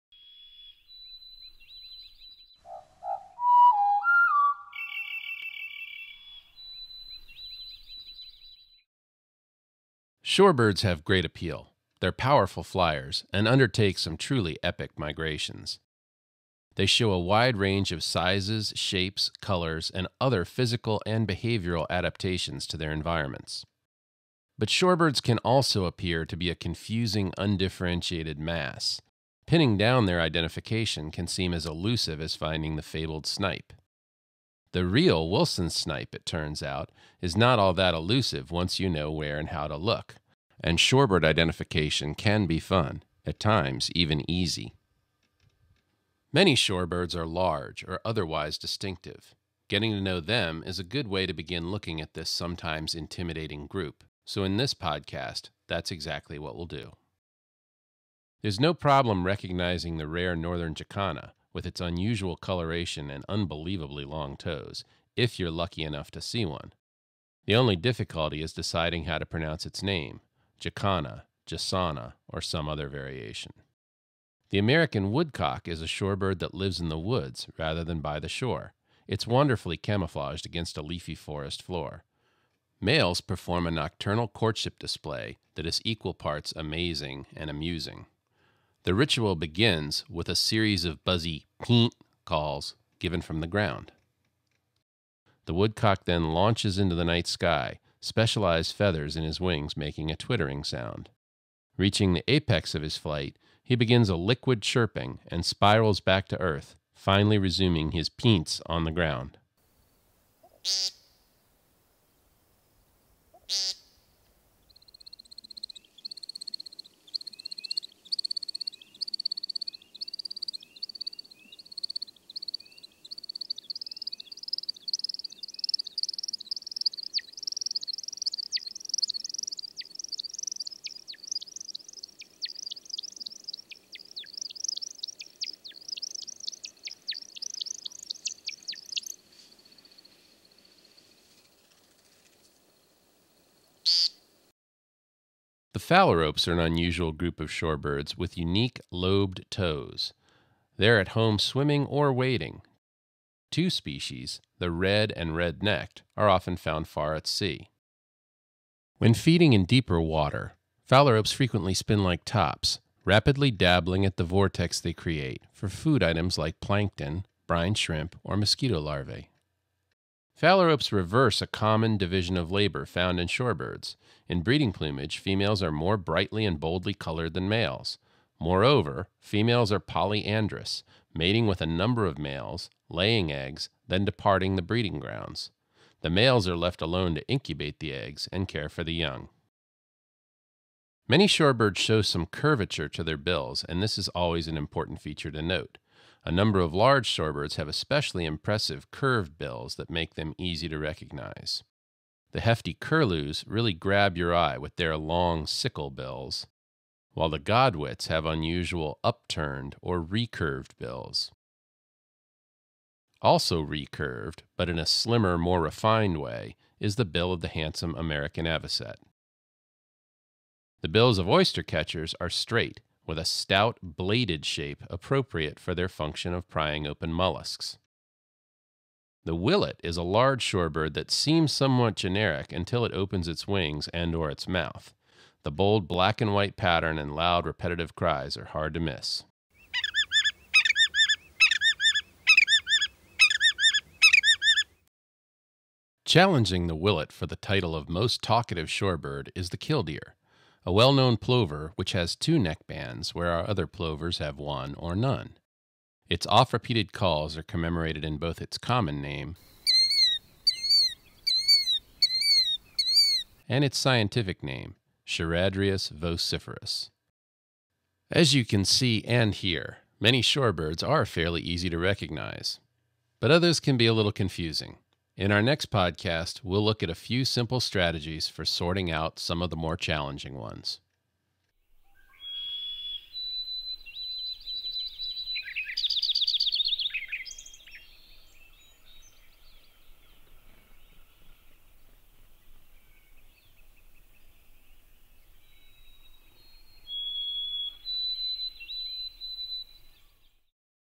Shorebirds Overview 2008
Shorebirds have a reputation for being difficult to identify, but in reality, the family has a number of distinctive members. This video looks at some of the standouts in the shorebird clan.